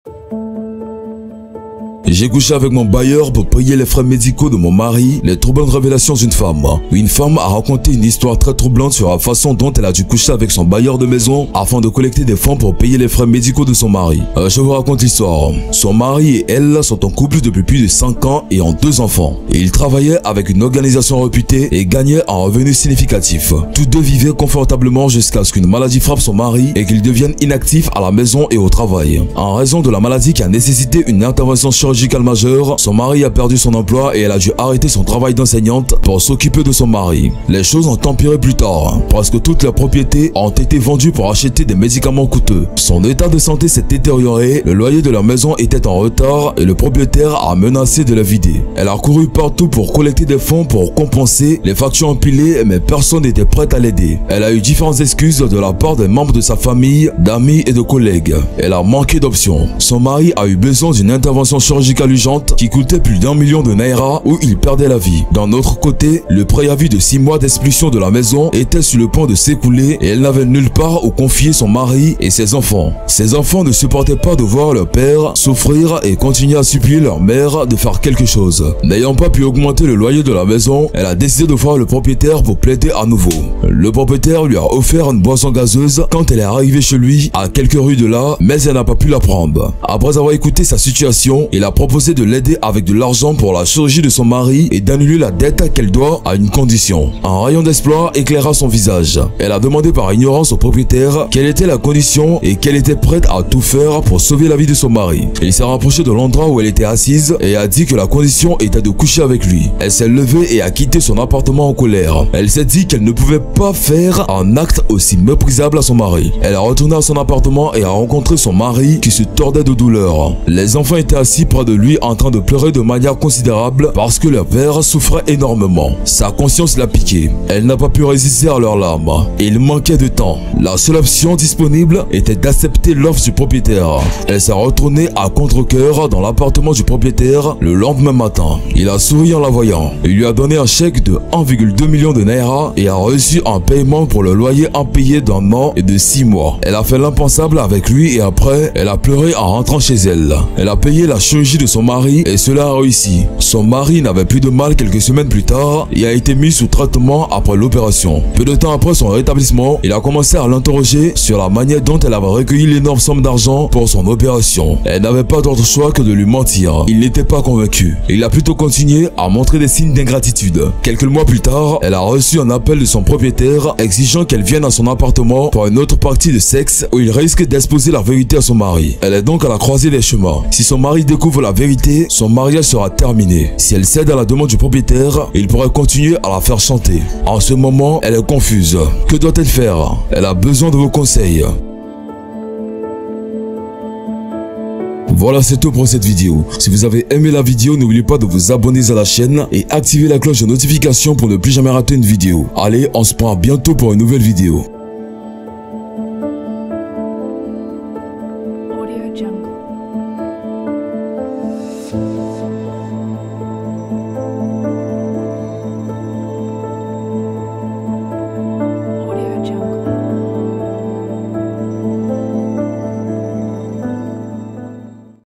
J’ai couché avec mon bailleur pour payer les frais médicaux de mon mari
« J’ai couché avec mon bailleur pour payer les frais médicaux de mon mari » les troublantes révélations d’une femme.

Une femme a raconté une histoire très troublante sur la façon dont elle a dû coucher avec son bailleur de maison afin de collecter des fonds pour payer les frais médicaux de son mari.